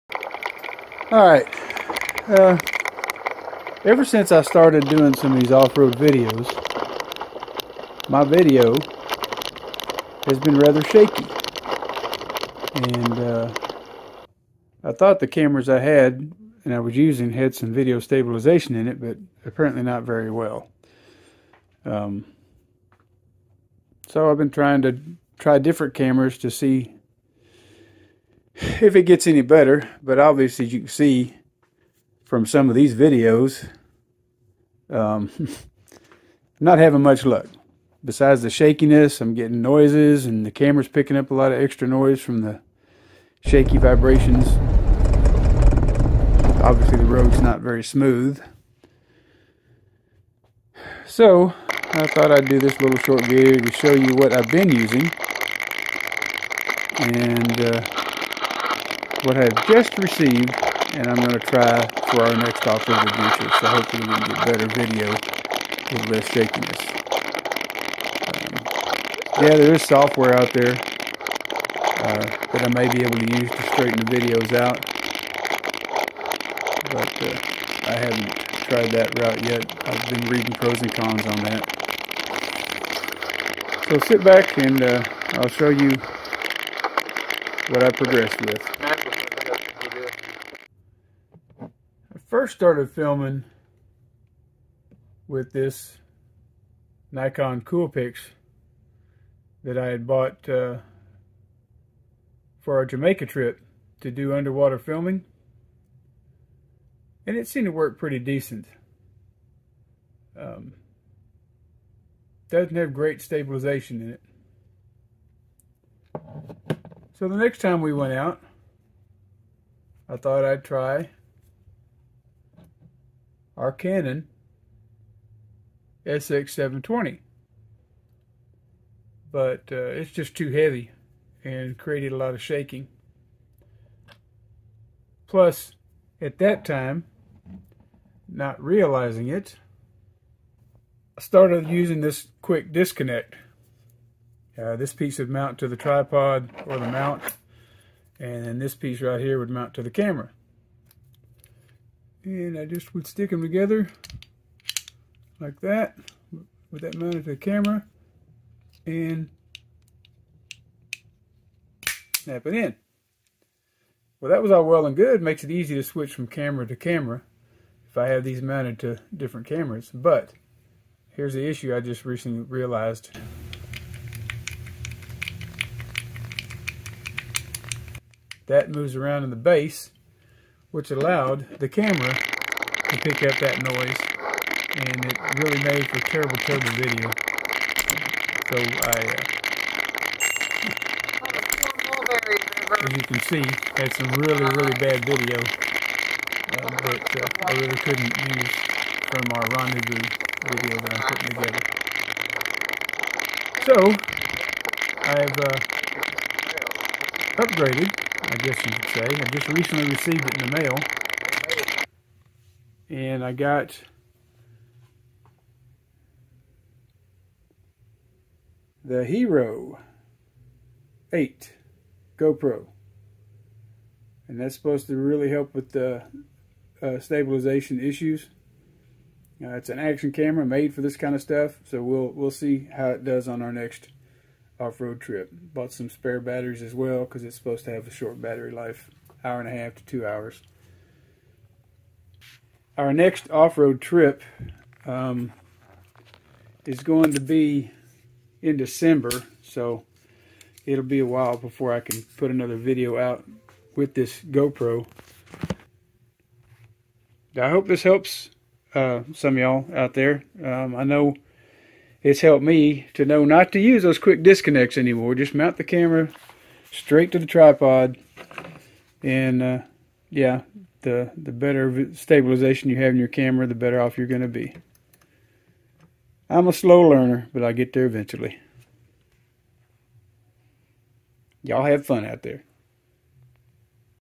Shakes and Jitters
Some issues I have encounter trying to video 4x4 off road excursions. The main problem is that I already owned some small cameras (1 waterproof) that I hesitated to get an action camera.